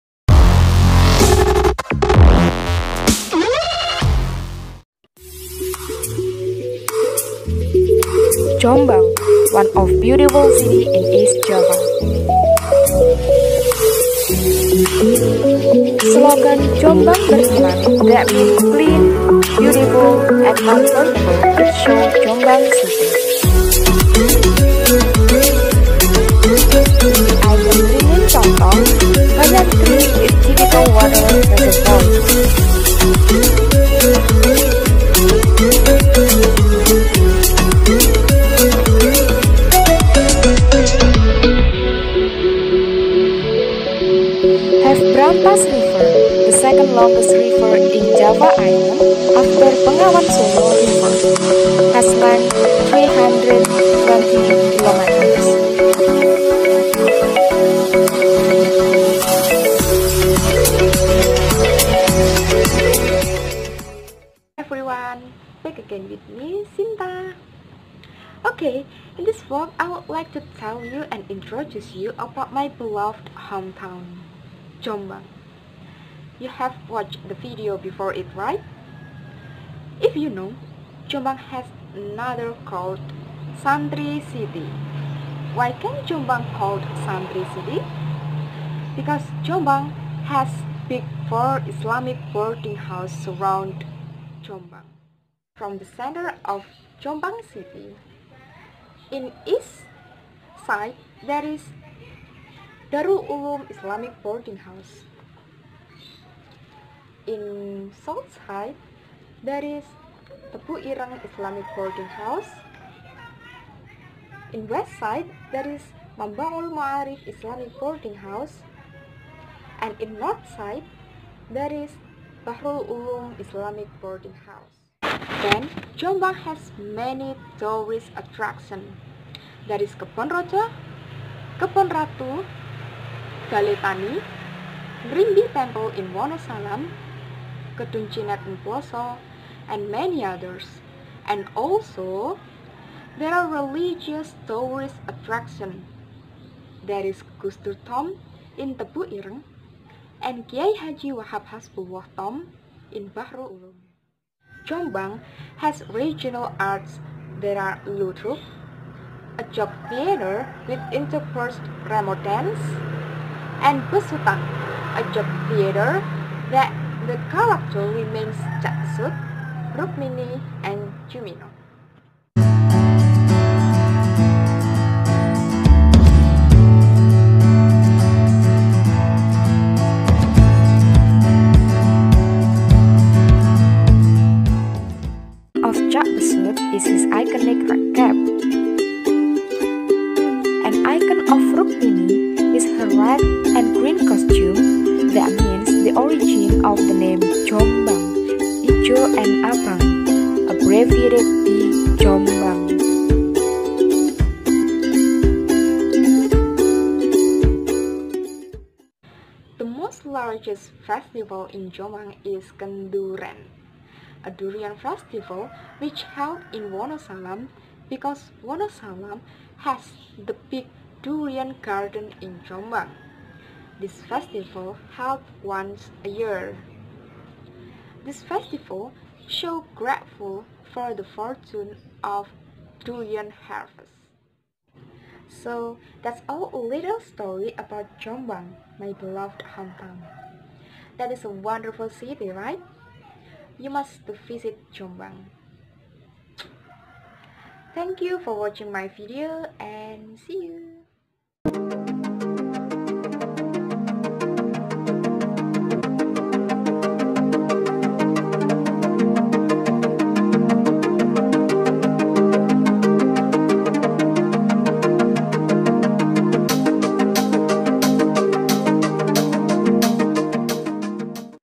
SINTA VLOG 3: INTRODUCE MY HOMETOWN
Jombang, one of beautiful city in East Java. Slogan “JOMBANG BERIMAN” that mean clean, beautiful, and comfortable it showed Jombang city. With the icon RINGIN CONTONG, banyan tree with typical water reservoir. Has Brantas river, the second longest river in Java Island after Bengawan Solo river, has length 320 km.
Hello everyone! Back again with me, Sinta. In this vlog I want to tell you and introduce you my hometown, Jombang. You have watched the video before it right? If you know, that Jombang has another called, “Santri city”. Why can Jombang called “Santri City”? Because Jombang has big 4 Islamic boarding house surround Jombang. From the center of Jombang city, in east side there is Darul Ulum Islamic Boarding House, in south side there is Tebu Ireng Islamic Boarding House, in west side there is Mamba’ul Ma’arif Islamic Boarding House, and in north side there is Bahrul Ulum Islamic Boarding House.
Jombang has many tourist attractions, there are Kebon Rojo, Kebon Ratu, Bale Tani, Ngrimbi Tample in Wonosalam, Kedung Cinet in Ploso and many others. And also there are religious tourist attraction, there are Gus Dur Tomb in Tebu Ireng, and KH. Wahab Hasbullah tomb in Bahrul Ulum.
Then Jombang has regional arts, there are Ludruk, a joke theater with interspersed with Remo dance, and Besutan, a joke theater that the character remains Cak Besut, Rukmini, and Jumino.
The icon of Cak Besut is his iconic red cap, and the icon of Rukmini is her red and green costume, that means the origin of the name Jombang (Ijo and Abang, abbreviated be Jombang)
The largest festival in Jombang is “KenDUREN”, a durian festival which held in Wonosalam because Wonosalam has the big durian garden in Jombang. This festival held once a year. This festival show grateful for the fortune of durian harvest.
So, that is all the little story about my beloved hometown, Jombang. That is a wonderful city right? You must visit Jombang.

COMMENT: The introduction has loud music that drowns your speech. The content is good enough, and you can make the images to work together with your explanation by keep playing the music, but in the background and less louder than your speech volume.